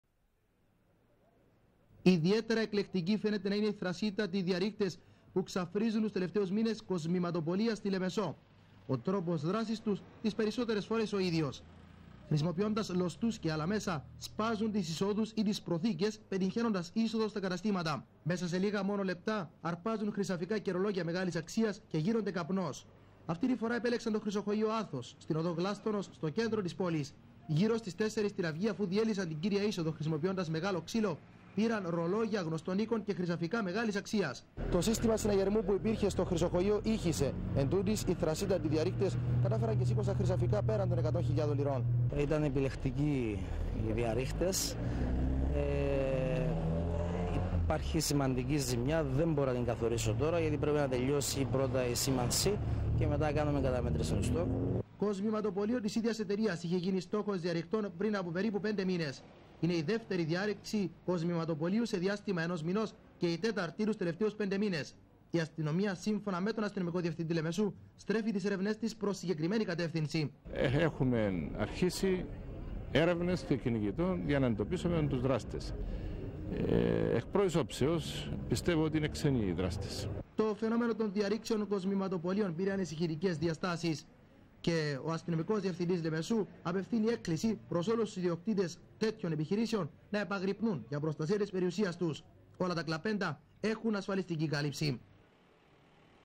Τον Σεπτέμβριο του 2002 έγινε διάρρηξη στο κοσμηματοπωλείο Athos
Ρεπορτάζ: Ευάγγελος Ευαγγέλου
Κάμερα: Κλείτος Ερωτοκρίτου
Μοντάζ: Ανδρέας Μιχαηλίδης
Παραγωγή: Videoscope studio